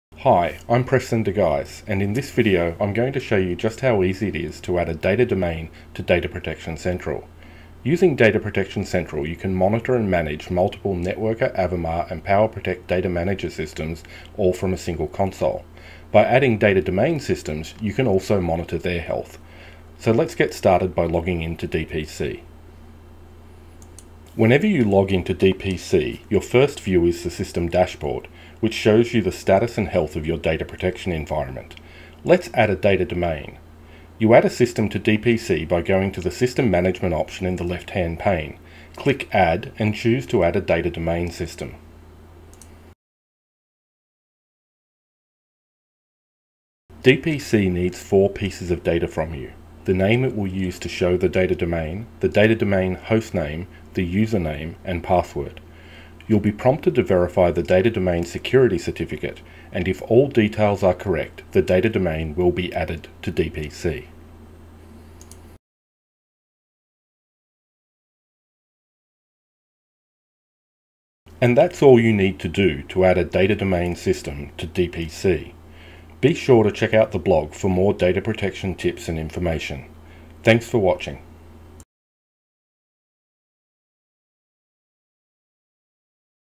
Adding a Data Domain system to Data Protection Central
This video shows how easy it is to add your Data Domain systems to Data Protection Central.

DPC is a central dashboard for your Dell Technologies data protection solutions: it can provide a single view over hundreds of Avamar, NetWorker, PowerProtect Data Manager Systems, and Data Domains. You can also integrate it with Data Protection Advisor (DPA) to run reports against all those systems from one place.